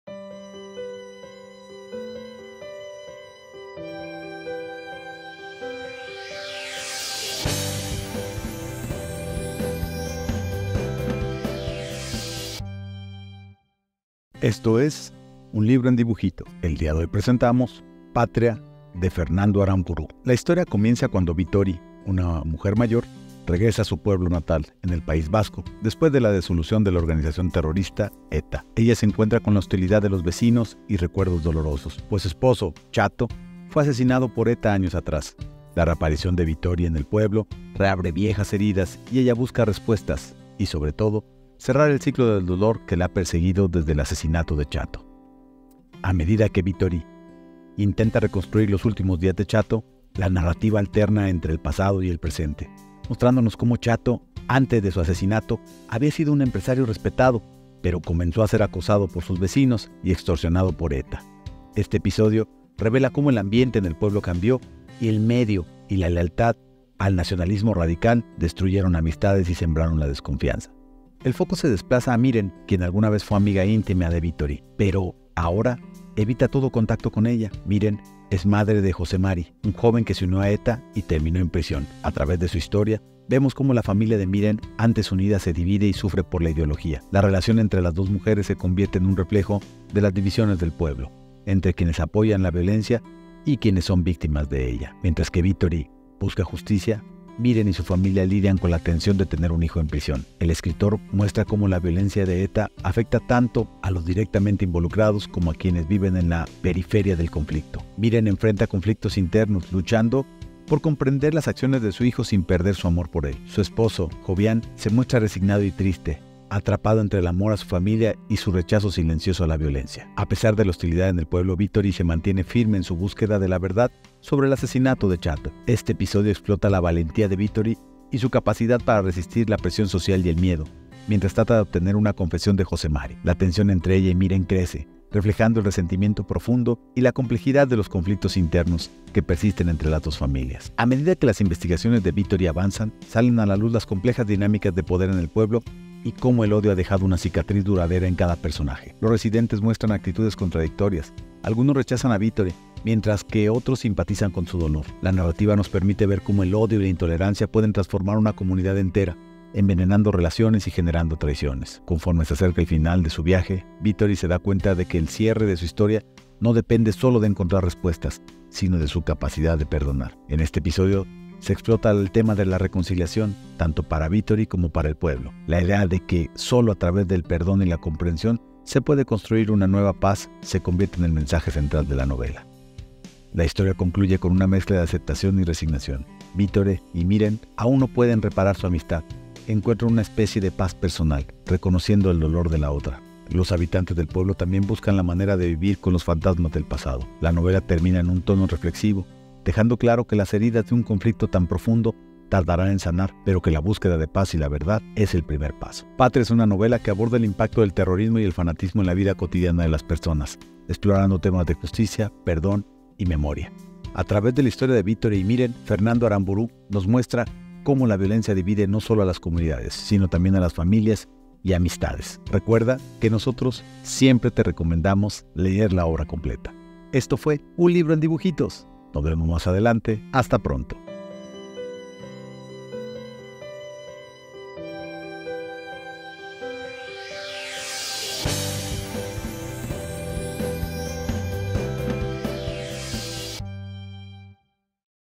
Patria - Fernando Aramburu - Un Libro en Dibujitos (resumen para estudiantes)
Episodio 1: “El regreso de Bittori”
La historia comienza cuando Bittori, una mujer mayor, regresa a su pueblo natal en el País Vasco después de la disolución de la organización terrorista ETA. Ella se encuentra con la hostilidad de los vecinos y recuerdos dolorosos, pues su esposo, Txato, fue asesinado por ETA años atrás. La reaparición de Bittori en el pueblo reabre viejas heridas, y ella busca respuestas y, sobre todo, cerrar el ciclo de dolor que la ha perseguido desde el asesinato de Txato.

Episodio 2: “El peso de la memoria”
A medida que Bittori intenta reconstruir los últimos días de Txato, la narrativa alterna entre el pasado y el presente, mostrándonos cómo Txato, antes de su asesinato, había sido un empresario respetado, pero comenzó a ser acosado por sus vecinos y extorsionado por ETA. Este episodio revela cómo el ambiente en el pueblo cambió, y el miedo y la lealtad al nacionalismo radical destruyeron amistades y sembraron desconfianza.

Episodio 3: “La familia de Miren”
El foco se desplaza a Miren, quien alguna vez fue amiga íntima de Bittori, pero que ahora evita todo contacto con ella. Miren es madre de Joxe Mari, un joven que se unió a ETA y terminó en prisión. A través de su historia, vemos cómo la familia de Miren, antes unida, se divide y sufre por la ideología. La relación entre las dos mujeres se convierte en un reflejo de las divisiones en el pueblo, entre quienes apoyan la violencia y quienes son víctimas de ella.

Episodio 4: “La violencia en casa”
Mientras que Bittori busca justicia, Miren y su familia lidian con la tensión de tener un hijo en prisión. Aramburu muestra cómo la violencia de ETA afecta tanto a los directamente involucrados como a quienes viven en la periferia del conflicto. Miren enfrenta conflictos internos, luchando por comprender las acciones de su hijo sin perder su amor por él. Su esposo, Joxian, se muestra resignado y triste, atrapado entre el amor a su familia y su rechazo silencioso a la violencia.

Episodio 5: “La resistencia de Bittori”
A pesar de la hostilidad en el pueblo, Bittori se mantiene firme en su búsqueda de la verdad sobre el asesinato de Txato. Este episodio explora la valentía de Bittori y su capacidad para resistir la presión social y el miedo, mientras trata de obtener una confesión de Joxe Mari. La tensión entre ella y Miren crece, reflejando el resentimiento profundo y la complejidad de los conflictos internos que persisten entre las dos familias.

Episodio 6: “Consecuencias del odio”
A medida que las investigaciones de Bittori avanzan, salen a la luz las complejas dinámicas de poder en el pueblo y cómo el odio ha dejado una cicatriz duradera en cada persona. Los residentes muestran actitudes contradictorias; algunos rechazan a Bittori, mientras que otros simpatizan con su dolor. La narrativa nos permite ver cómo el odio y la intolerancia pueden transformar una comunidad entera, envenenando relaciones y generando traiciones.

Episodio 7: “El perdón y la reconciliación”
Conforme se acerca al final de su viaje, Bittori se da cuenta de que el cierre de su historia no depende solo de encontrar respuestas, sino de su capacidad de perdonar. Este episodio explora el tema de la reconciliación, tanto para Bittori como para el pueblo. La idea de que solo a través del perdón y la comprensión se puede construir una nueva paz se convierte en el mensaje central de la novela.

Episodio 8: “El final de un ciclo”
La historia concluye con una mezcla de aceptación y resignación. Bittori y Miren, aunque no pueden reparar su amistad, encuentran una especie de paz personal, reconociendo el dolor de la otra. Los habitantes del pueblo también buscan la manera de vivir con los fantasmas del pasado. La novela termina en un tono reflexivo, dejando claro que las heridas de un conflicto tan profundo tardarán en sanar, pero que la búsqueda de paz y verdad es el primer paso.

Reflexión final para estudiantes:
Patria es una novela que aborda el impacto del terrorismo y el fanatismo en la vida cotidiana de las personas, explorando temas de justicia, perdón y memoria. A través de la historia de Bittori y Miren, Fernando Aramburu nos muestra cómo la violencia divide, no solo a las comunidades, sino también a las familias y amistades. La novela invita a reflexionar sobre el costo del odio y la importancia del perdón, mostrando que, aunque la paz sea un camino difícil, es necesario recorrerlo para sanar las heridas de un pasado doloroso.